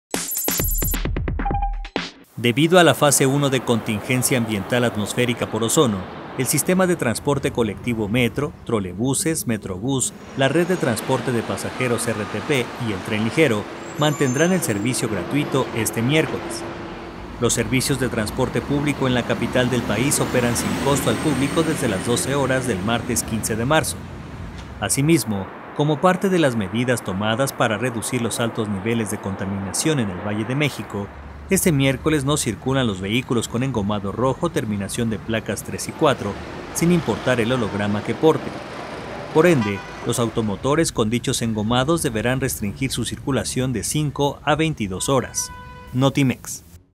Siguen Metro y Metrobús gratuitos por contingencia ambiental
Debido a la Fase I de Contingencia Ambiental Atmosférica por Ozono, el servicio de transporte en la CDMX será gratuito